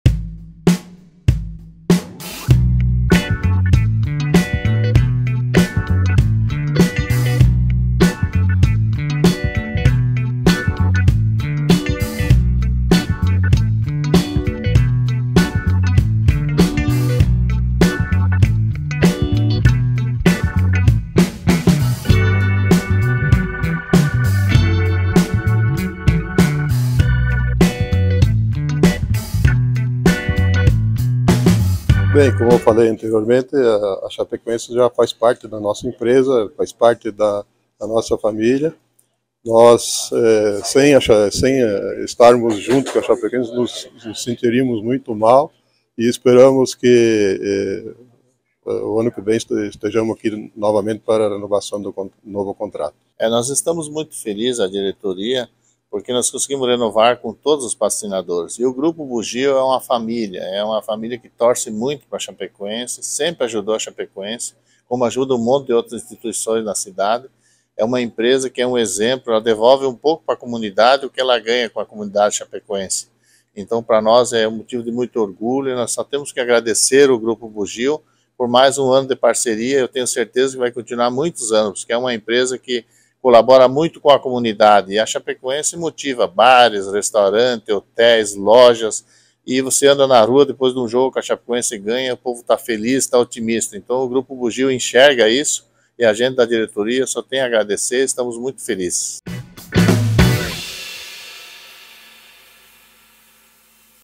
JUNTOS POR MAIS UMA TEMPORADA: GRUPO BUGIO PATROCINA O VERDÃO EM 2024
Na manhã desta quinta-feira (25) a Chapecoense anunciou a renovação contratual de patrocínio com o Grupo Bugio para a temporada 2024. Assista!

Imagens e edição: Tiago Meneghini/João Vitor Heemann/ChapeTV/Unochapecó

Siga a Chape nas redes!